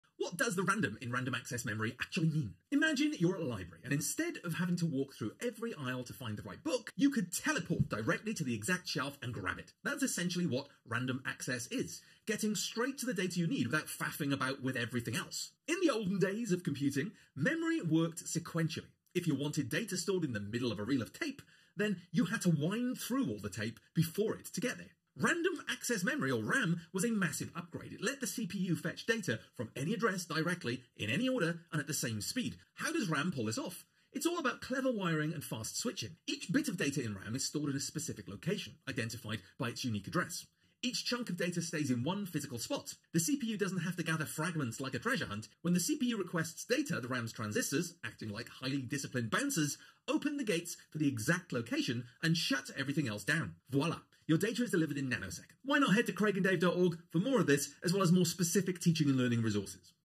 What does the Random in RAM actually mean?
Ever wondered what the "random" in Random Access Memory (RAM) actually means?
In this video, The Craig'n'Dave Lesson Hacker breaks down how RAM lets your computer access data in a flash, without all the hassle of waiting in line. 🚀
Curious to learn more about how RAM works its magic? Hit play and find out how this little piece of hardware makes your computer run faster than you could imagine!